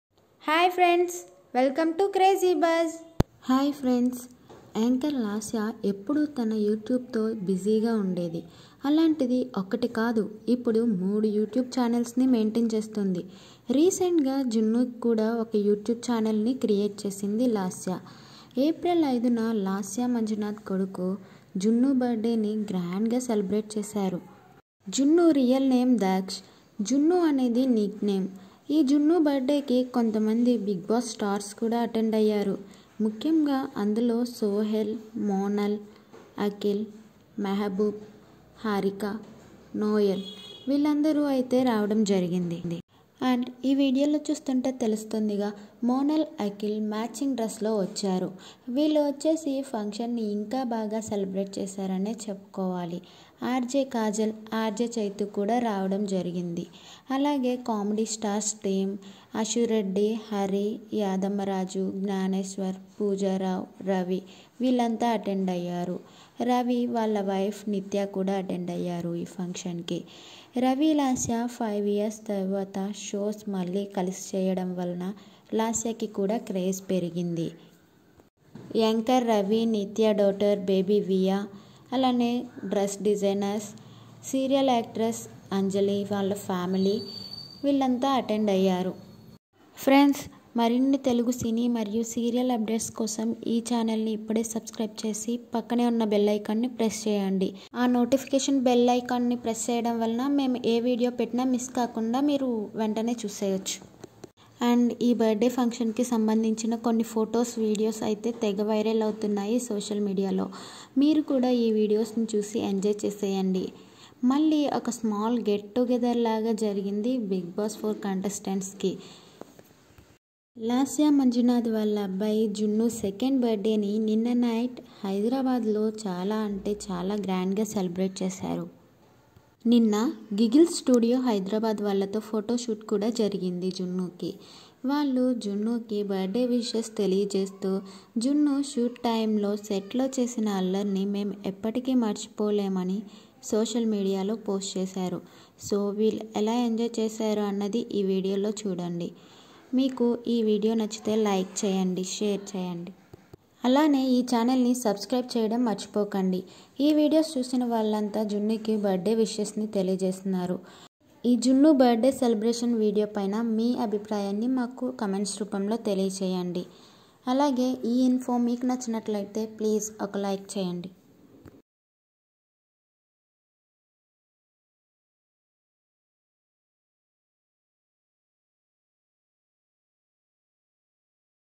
Lasya Son Junnu birthday party yesterday night at hyd ||Biggboss4 contestants hawa😍🙌Daksh​
Watch Lasya Son Junnu birthday party yesterday night at hyd ||Biggboss4 contestants hawa